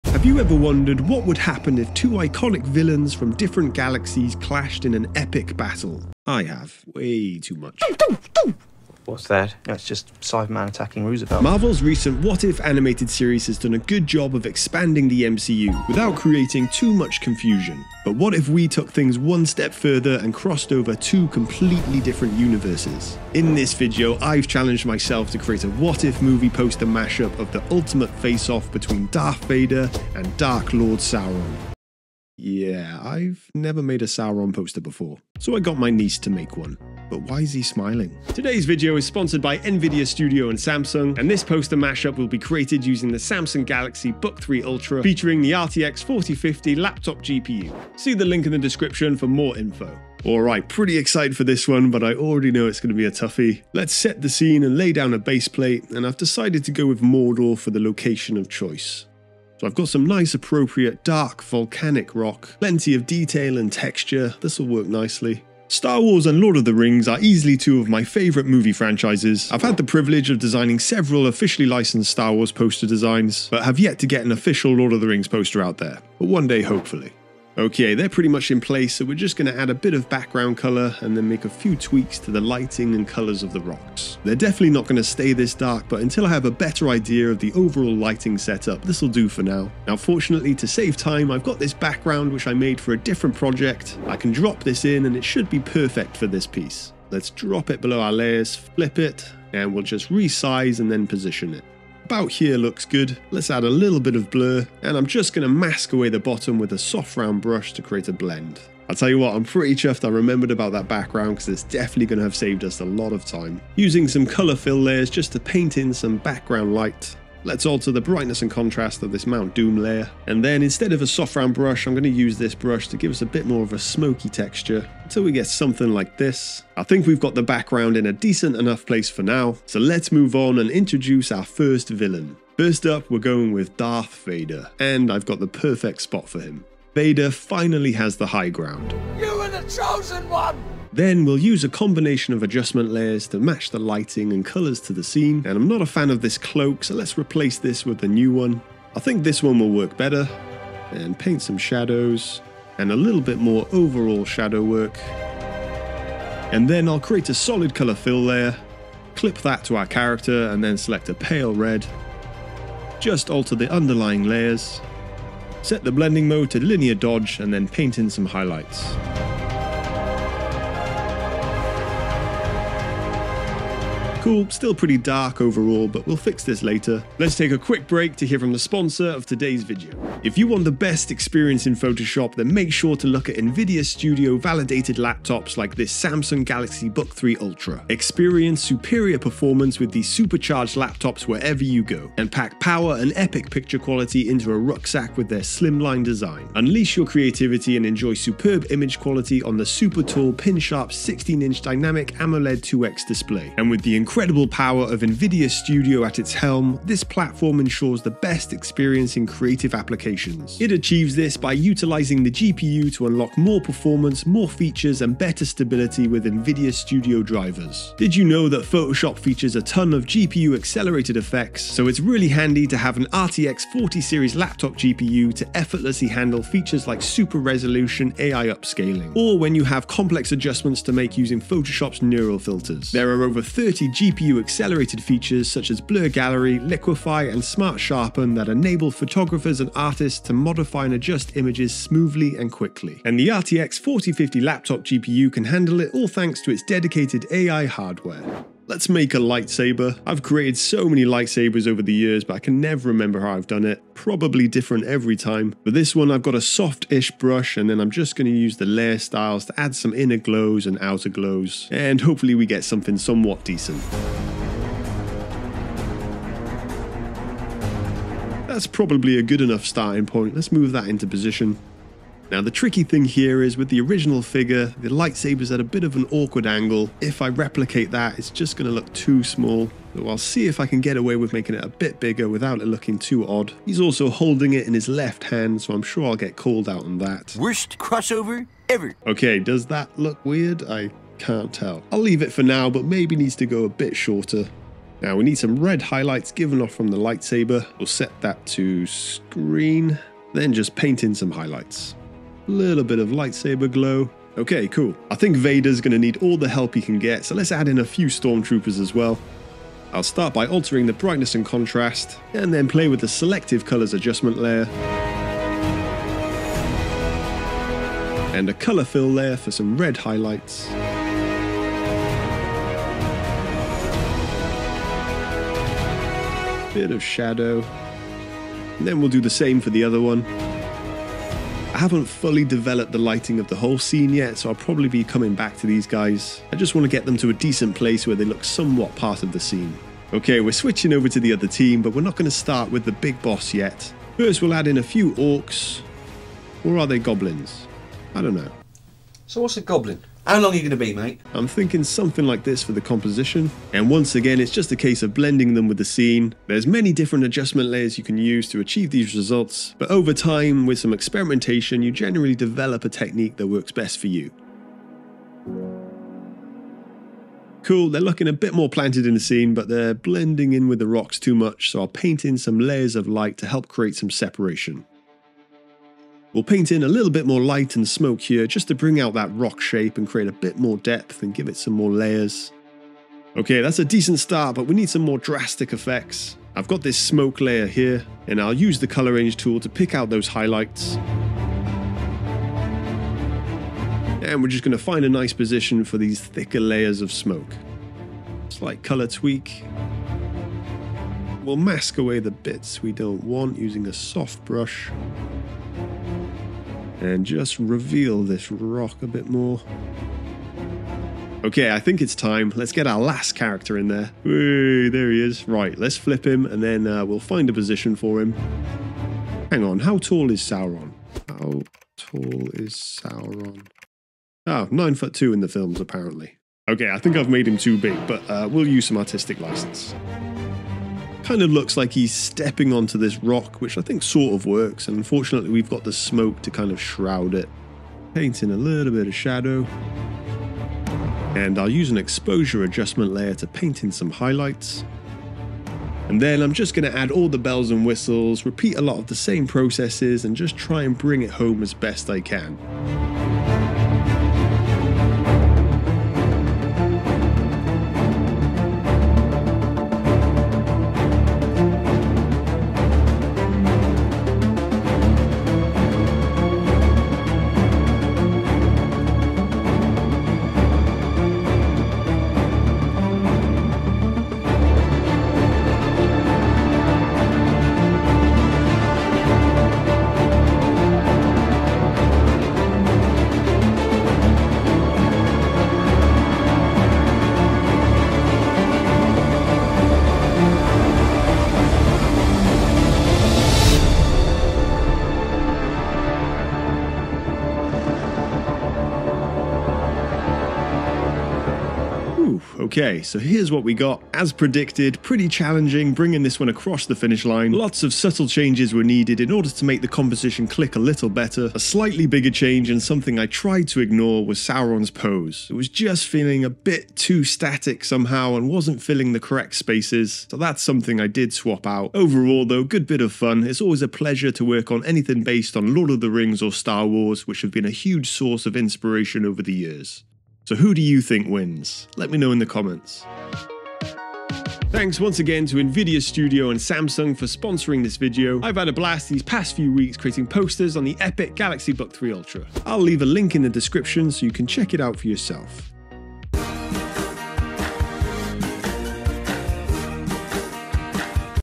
I Combined Two Famous Movies to Make One Epic Poster!
In this video, Star Wars crash-lands in Middle-earth, setting the stage for the ultimate face-off between Darth Vader and Sauron in this epic movie poster crossover

00:00 Introduction
00:47 Poster Creation
08:13 Final Poster Reveal